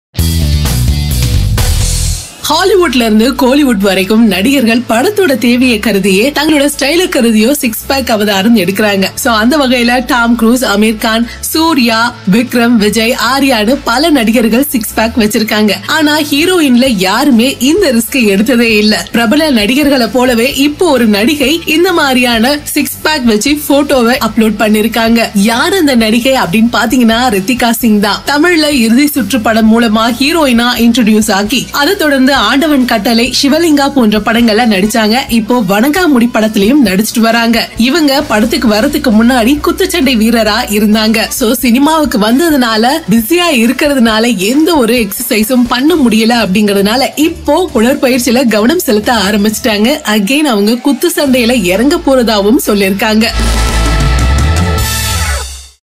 Rithika Singh Reveals Her Six Pack Abs | Kollywood Latest Gossip 2018
Actress Rithika Singh has got six pack abs. She revealed her six pack pic recently.